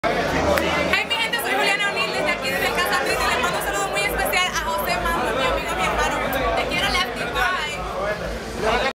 Juliana Oneal La Reina Del Mambo En Gala De Nominacion Premios Casandra 2011 JoseMambo.com
Juliana Oneal La Reina Del Mambo En Gala De Nominacion Premios Casandra 2011

Pagina Oficial Del Merengue 100% Internacional

Representante Oficial Steel Flow Latino